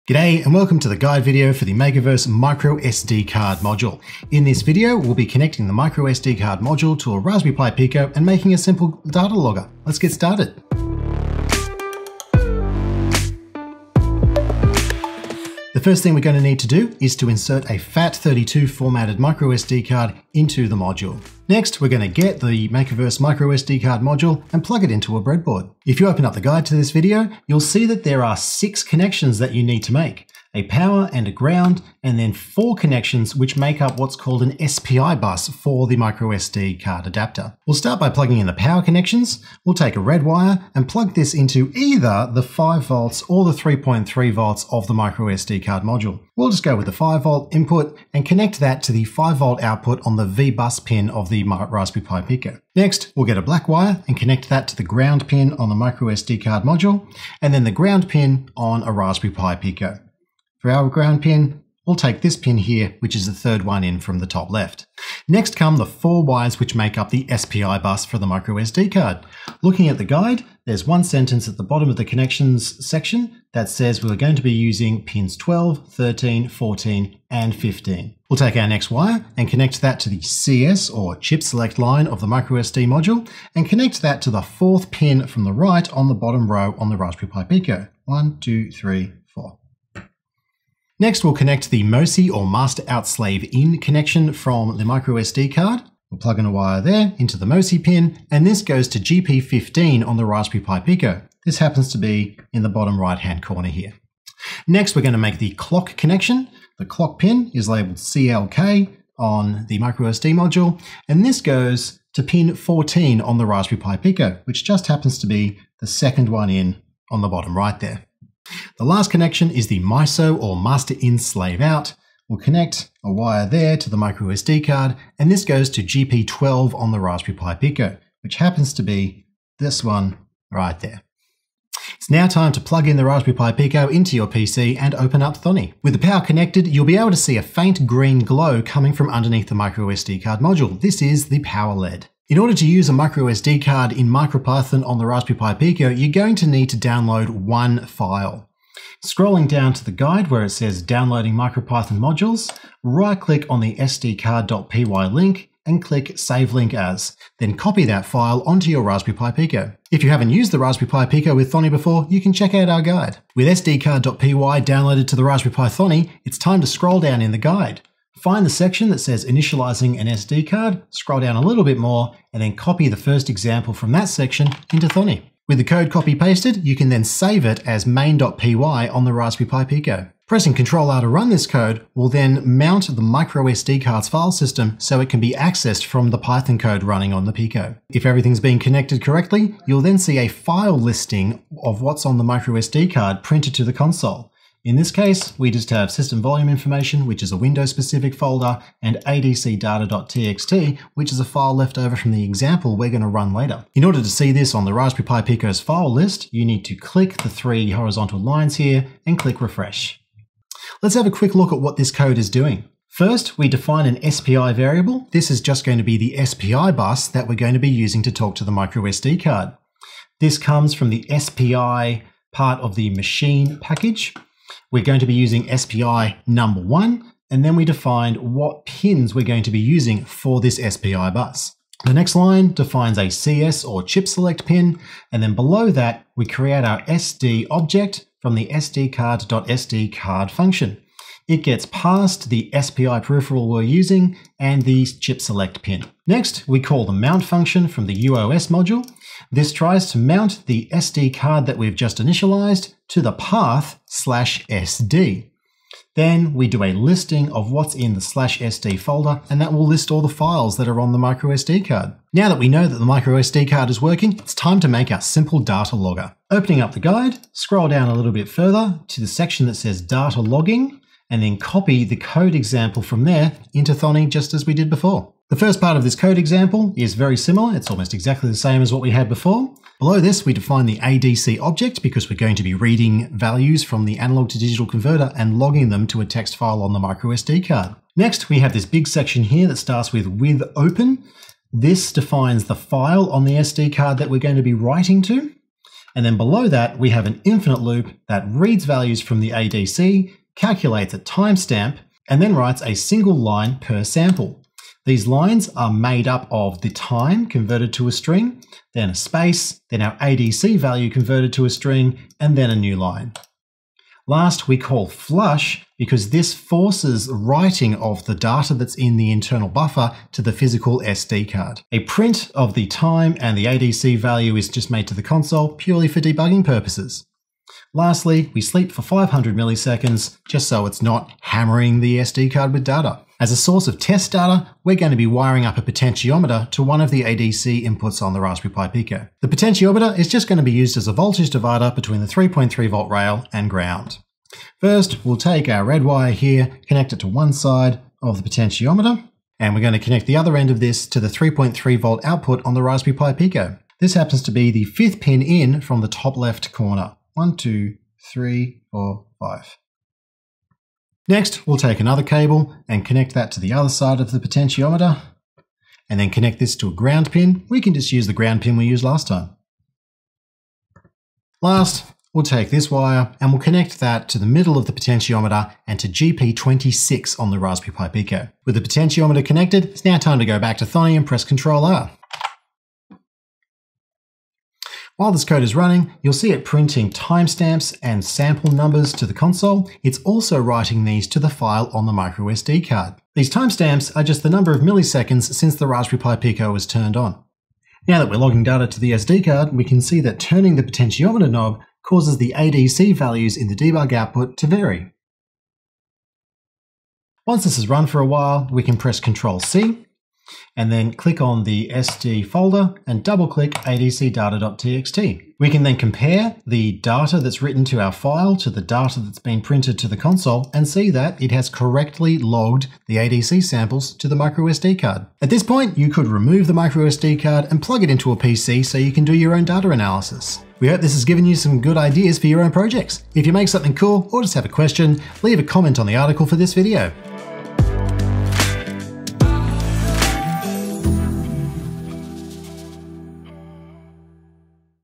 Makerverse Micro SD Adapter | Quickstart Guide with Raspberry Pi Pico
Let's get started using a Makerverse MicroSD Adapter - we'll connect it to a Raspberry Pi Pico and begin loggin some data collected by the ADC. Of course, from here you could log any data you like!